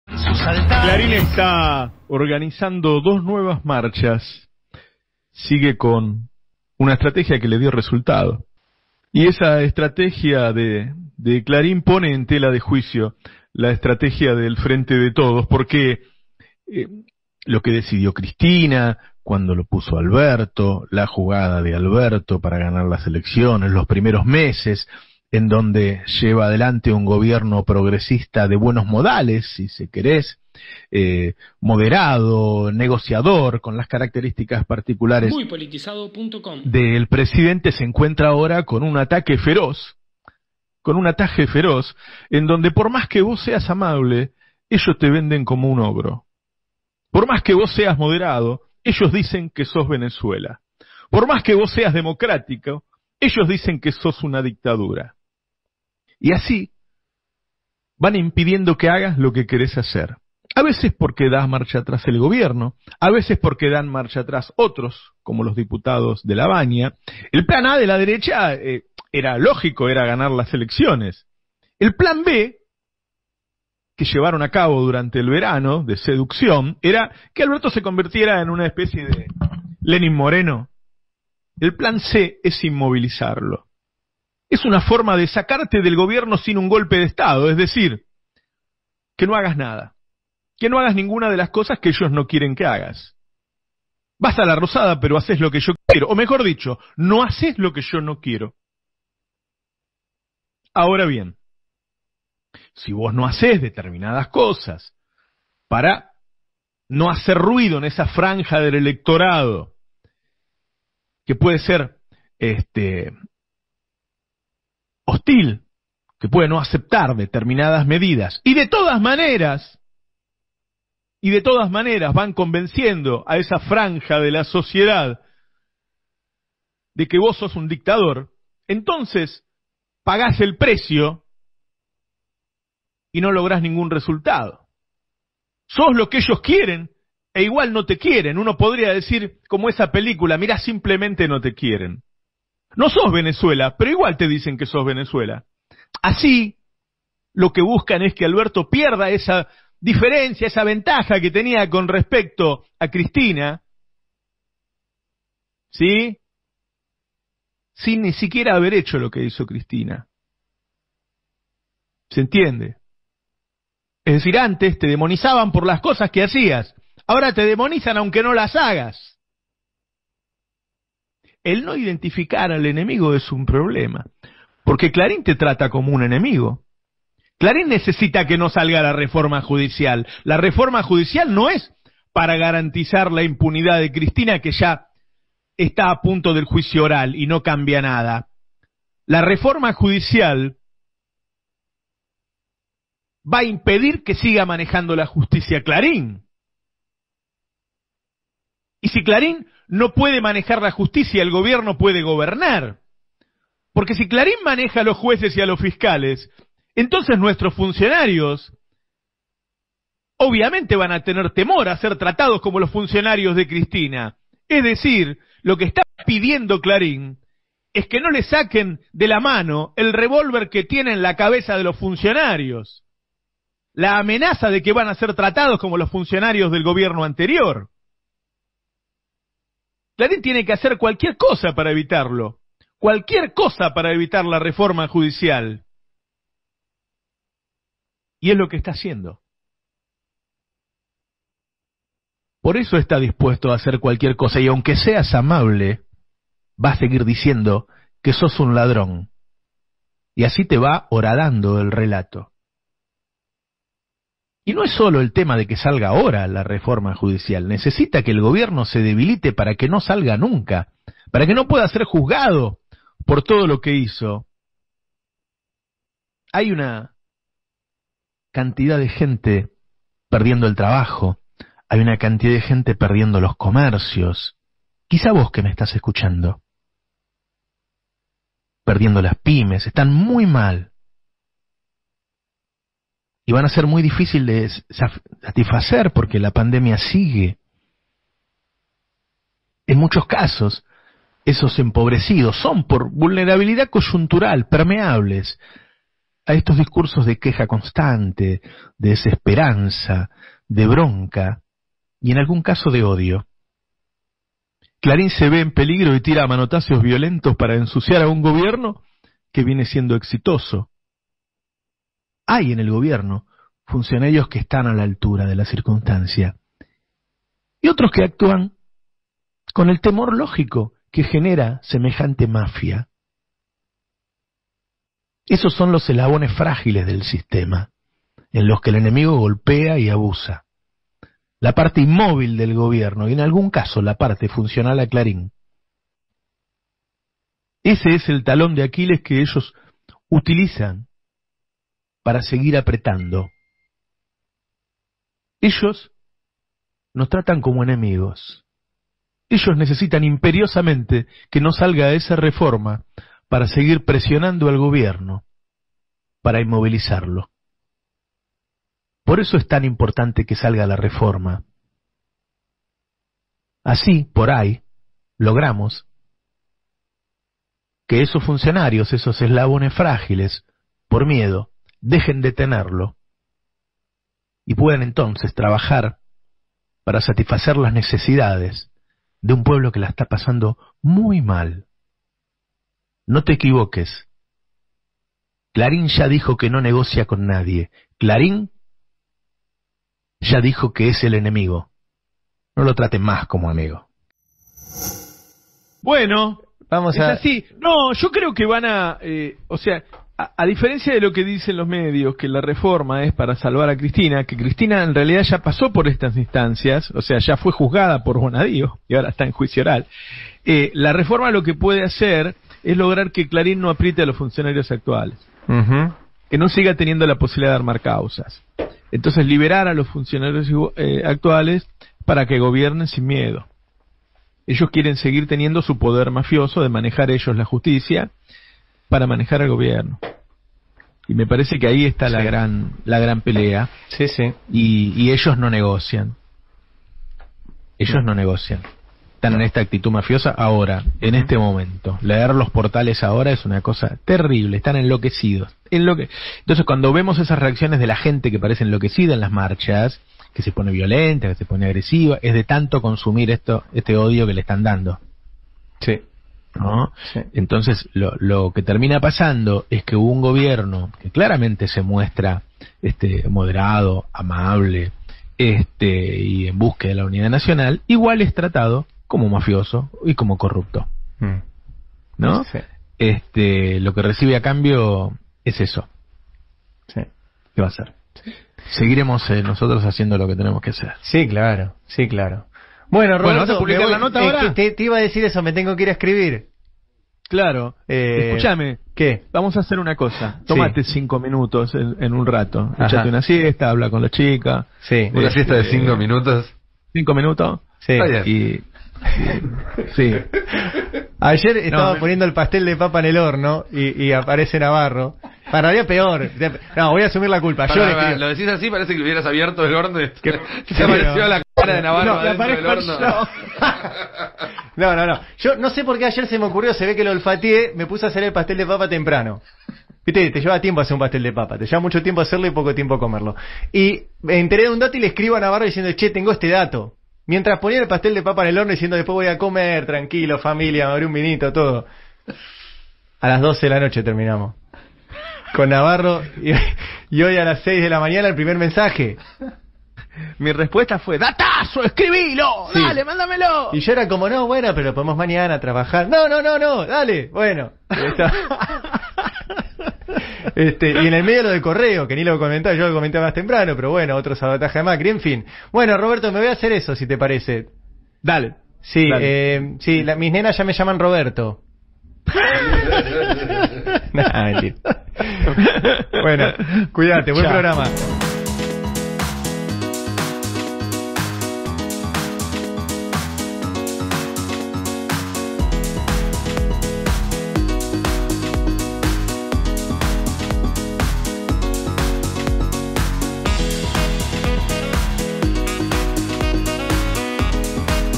POR ESTO ES IMPORTANTE LA REFORMA JUDICIAL | Editorial de Navarro
El editorial de hoy de Roberto Navarro estuvo referido a la importancia de la reforma judicial: "Clarín está organizando dos nuevas marchas", advierte. Escuchalo, es necesario.